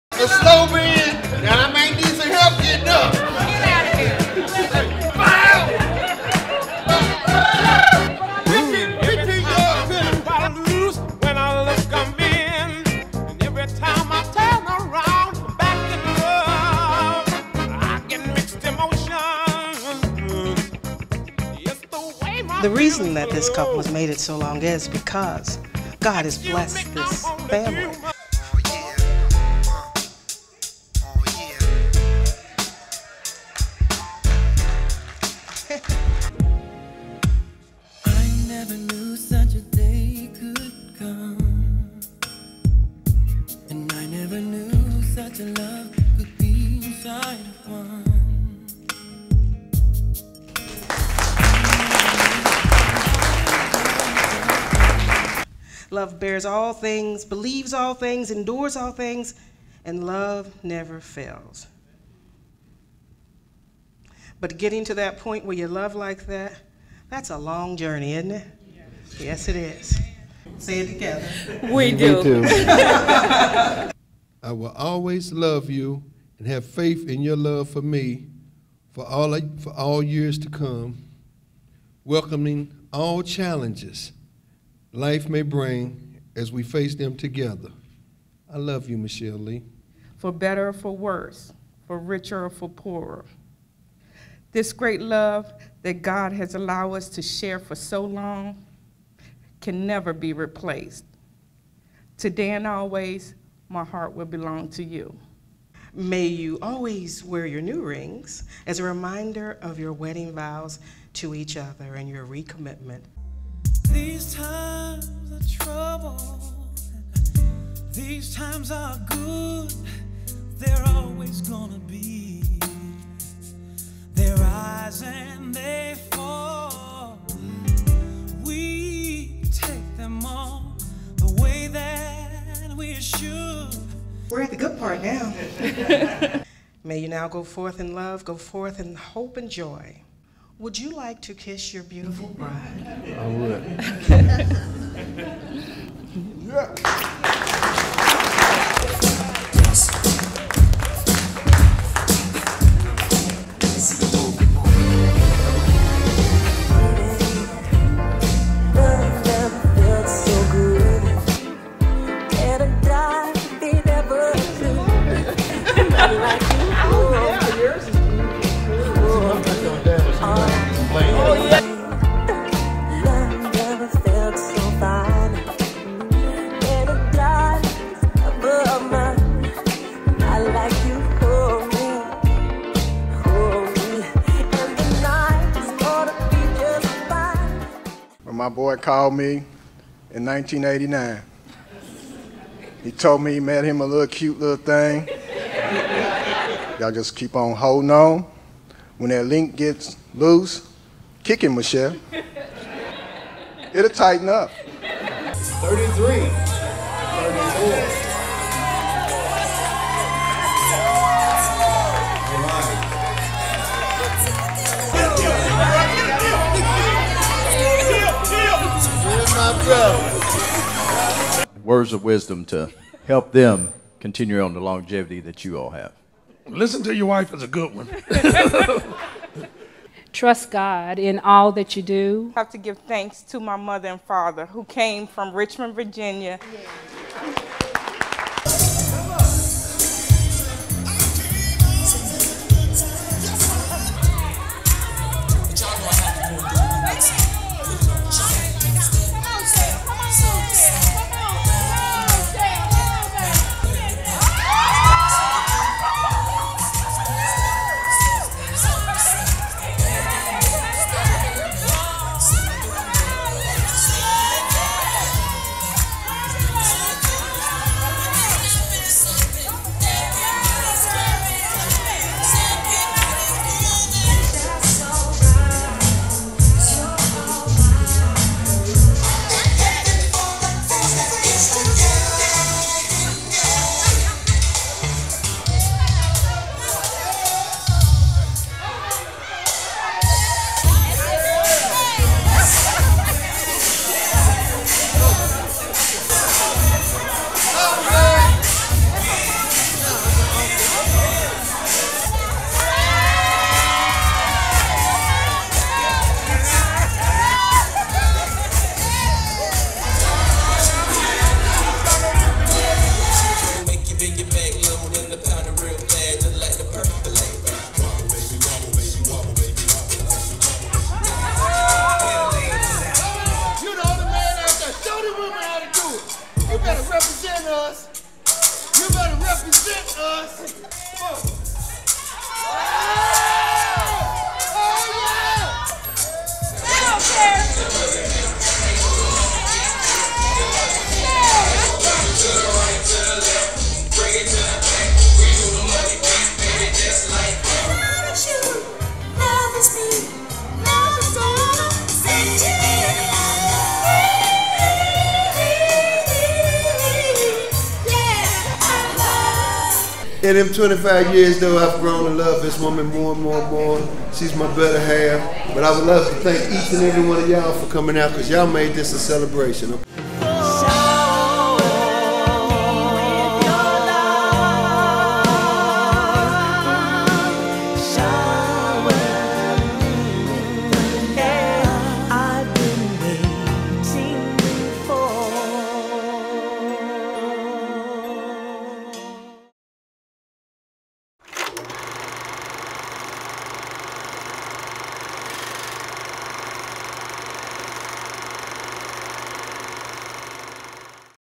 25th Anniv. Vow Renewal CER & PARTY Highlights
This was a beautiful event and you can really "feel the love" in this one.